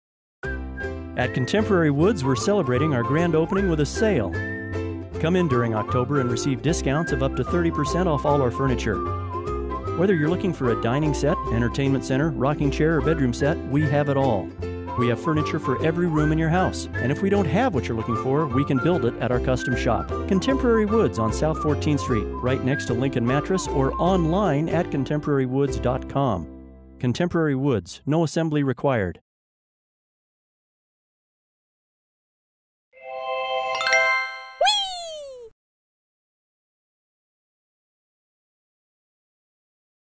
Contemporary Woods - "Grand Opening Sale"
This commercial was made for Contemporary Woods. They wanted to highlight the new sales they were having for their grand opening after a change in location from a previous store.